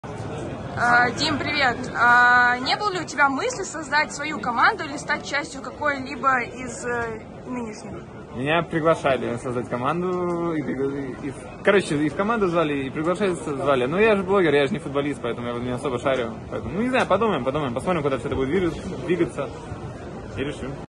🤔ДИМА ГОРДЕЙ СОЗДАЛ КОМАНДУ?!🔥
⚡️«Мысль создать свою команду? Меня и приглашали создать команду, и звали в одну из, но я же блогер, я не футболист и в этом не особо шарю. Подумаем, посмотрим куда это будет двигаться и решим, пока мне нравится быть как болельщик» — Дима Гордей